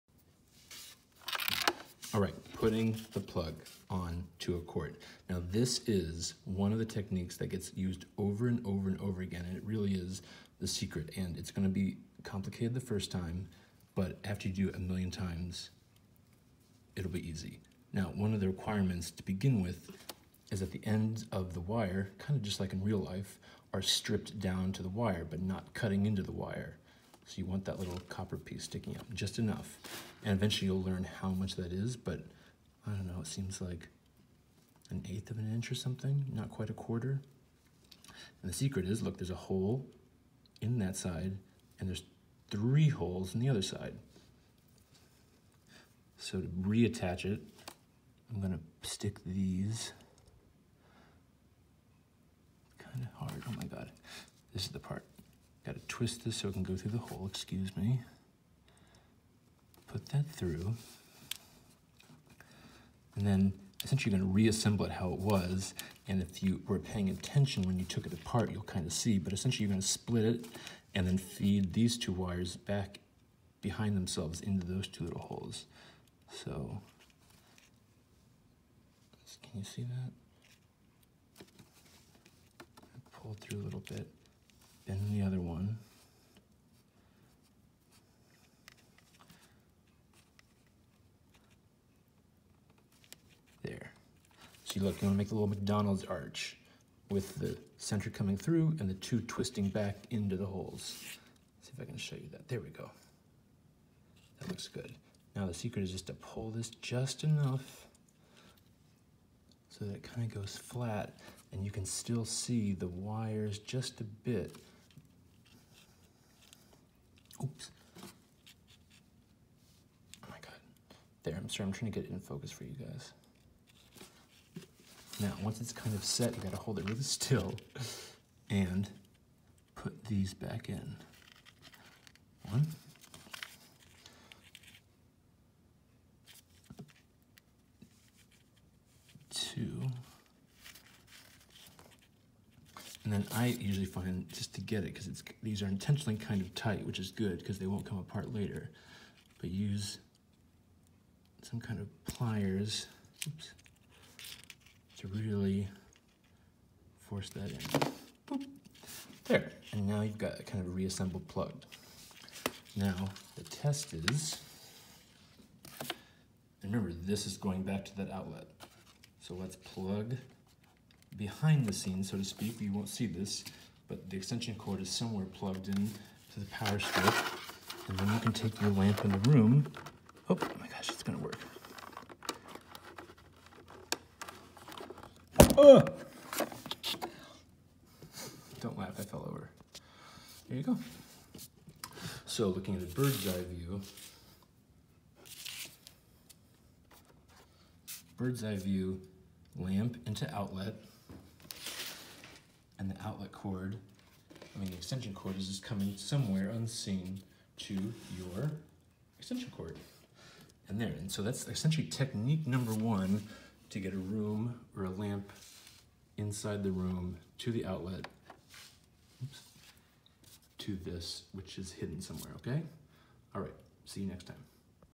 #4 - Dollhouse electrical 'Wiring 101', (re)threading wires into plug
Threading and re-threading the electrical wires into the plugs is a foundational skill, and will be used over and over again in the electrification of your house or boombox. But like riding a bike, once you get a hold of this tiny but important technique you will be able to tackle almost any miniature electrical project.